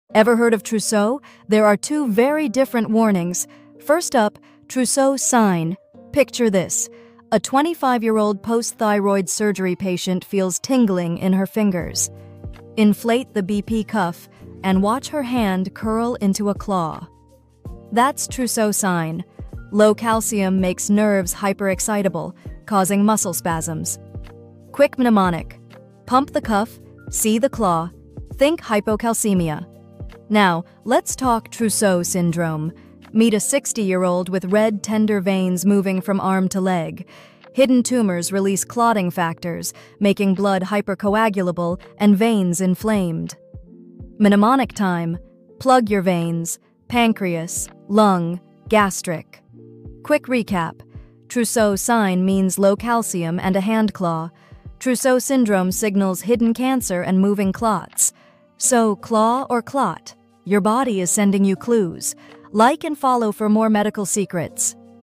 “Trousseau Sign vs Trousseau Syndrome: Claw or Clot? ⚡🩸 | Know the Secret Warning from Your Body!”
Description:
“Ever noticed your hand curling into a claw or veins mysteriously moving across your body? 🐾🩸
Discover the difference between Trousseau Sign (hypocalcemia) and Trousseau Syndrome (cancer) in just 55 seconds!

✅ Learn how and why it happens
✅ Easy-to-understand mechanisms
✅ Quick mnemonics to remember for exams or real-life cases
✅ Case scenarios make it super relatable

Mnemonics included:

Trousseau Sign: Pump cuff → Hand Claw → Low Calcium

Trousseau Syndrome: PLuG your veins → Pancreas, Lung, Gastric

Watch till the end to spot the clues your body is giving you!